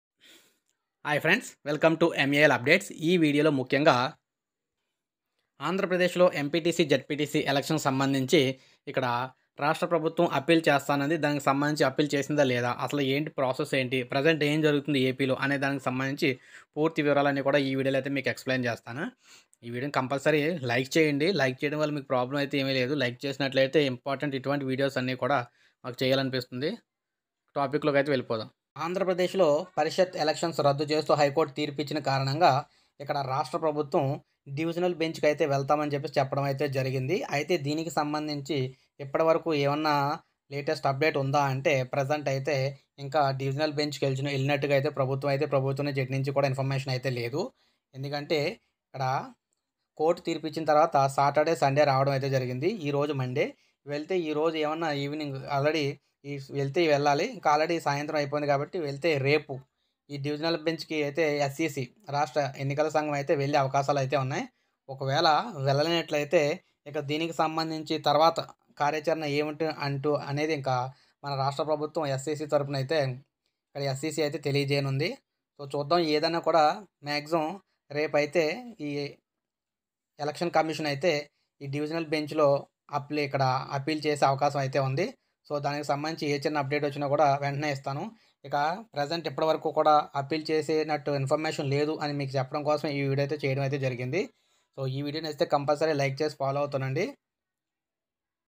latest update on MPTC, ZPTC elections ఏపీ ప్రభుత్వం డివిజనల్ బెంజ్ కి అప్లై చేసిందా లేదా.
Hello everyone today update is AP government latest update on MPTC, ZPTC elections appeal update.@malupdates